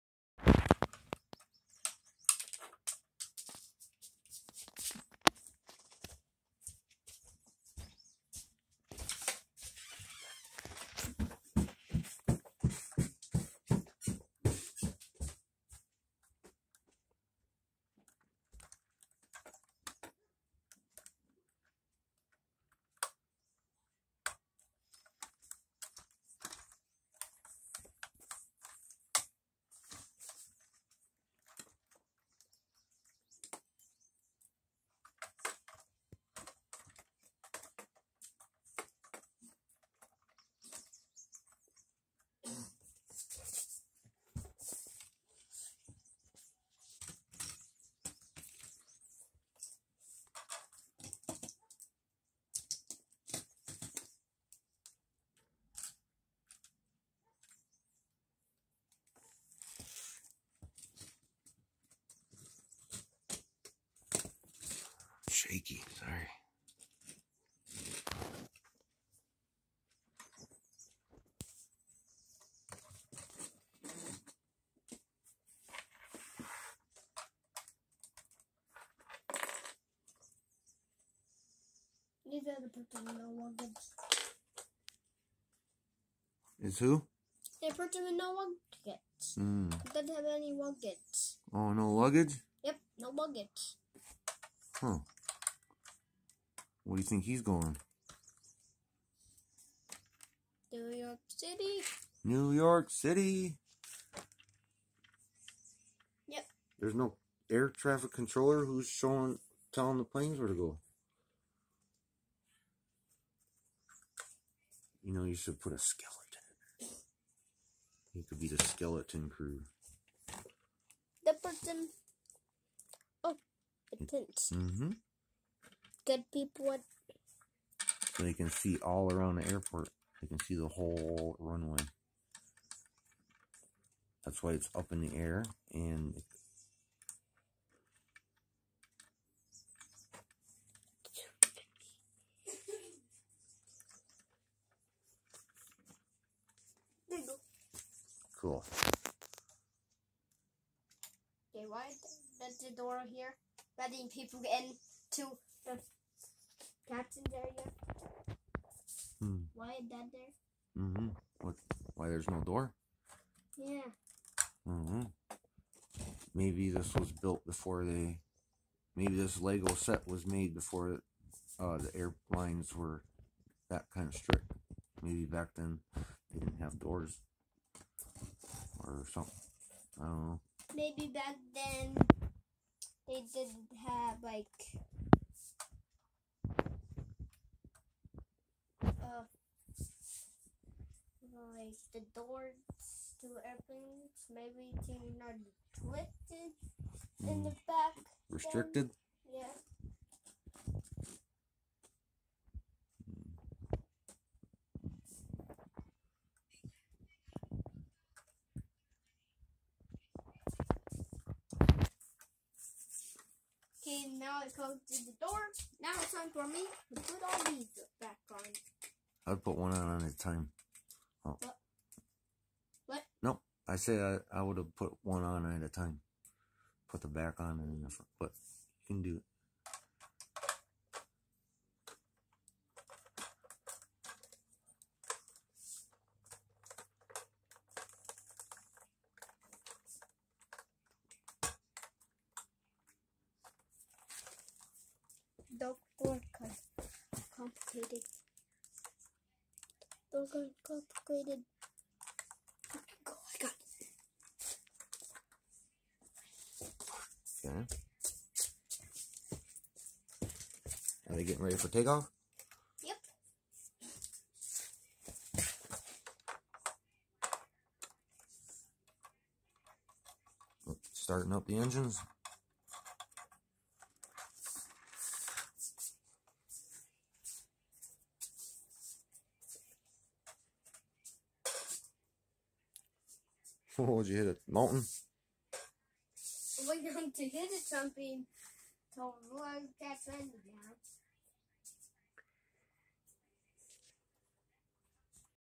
Arrow FINALLY gets his Lego Airport set
After almost 2 years we found the Lego Airport set. He was sooo excited.